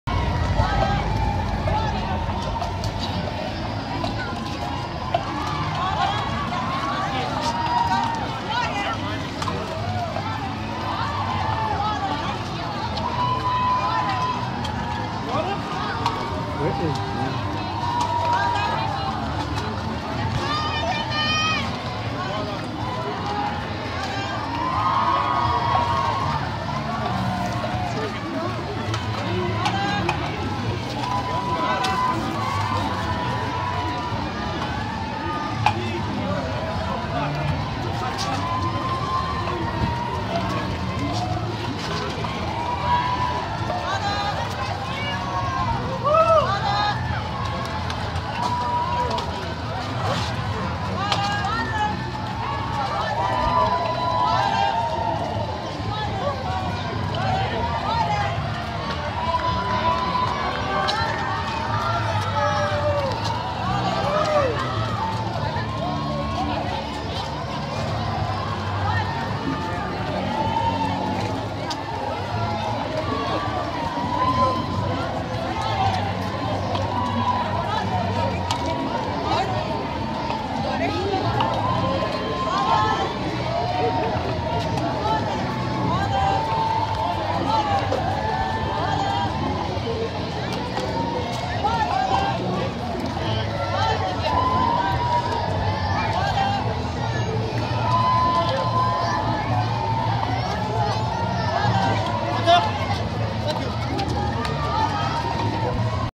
TCS NYRR 5th Mile Sunset Park Brooklyn Fluid Station
Was given the opportunity to be a volunteer at the TCS NYRR Marathon 5th Mile fluid’s station.